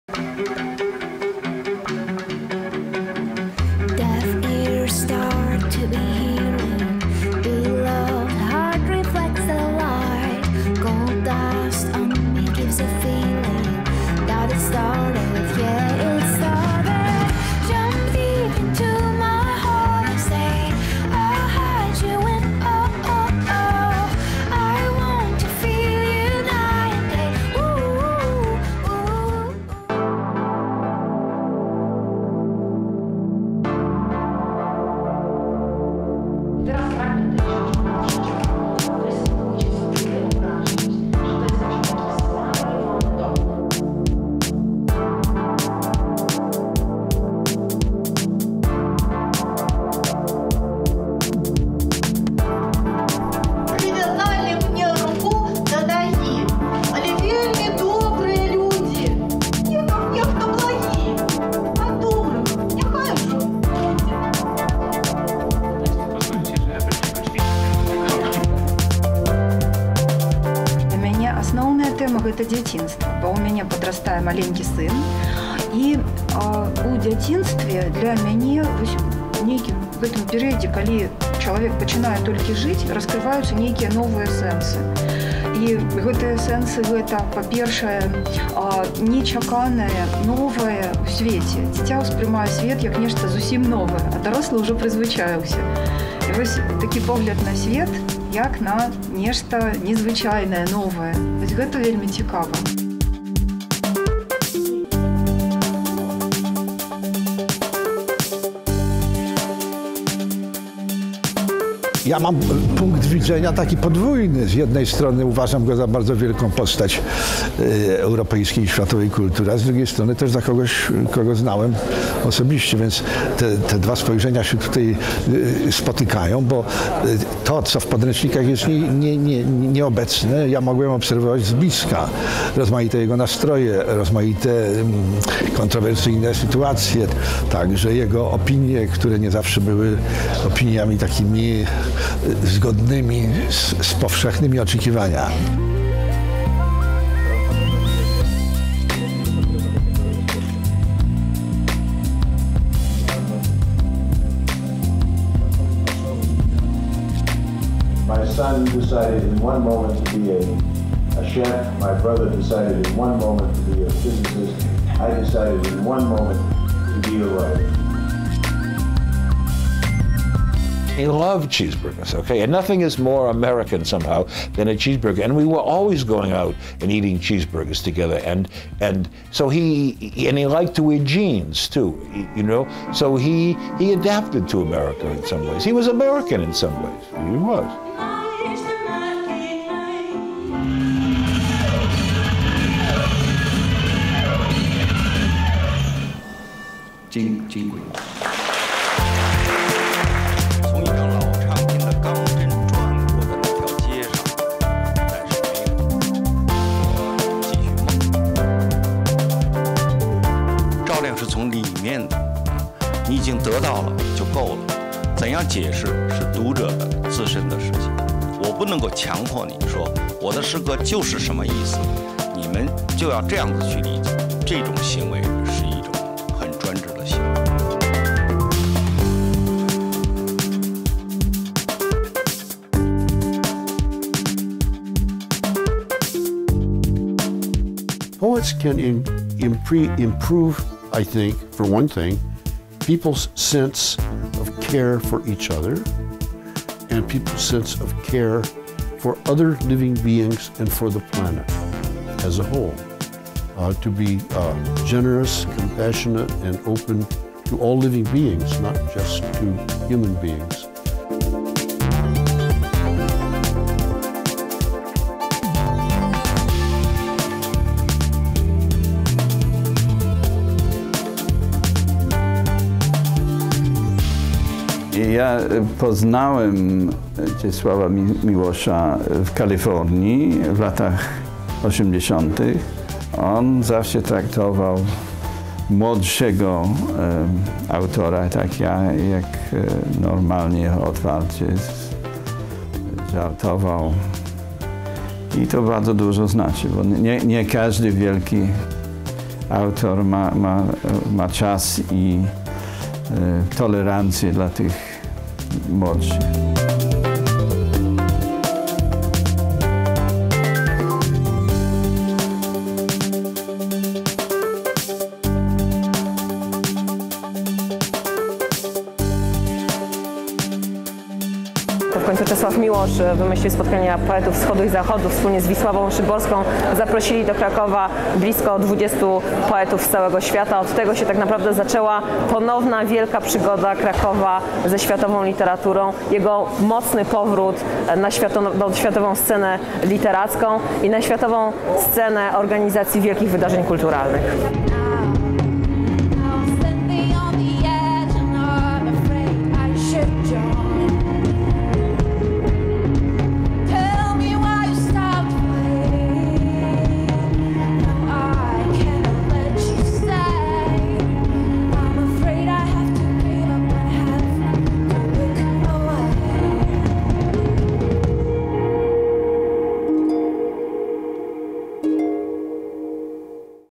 3rd Milosz Festival - reportage, part 2
16-19 May 2013, Krakow

Interviews with Duo Duo, Richard Lourie, Wiera Burłak, Gary Snyder, Norman Davies, Stefan Chwin and Magdalena Sroka.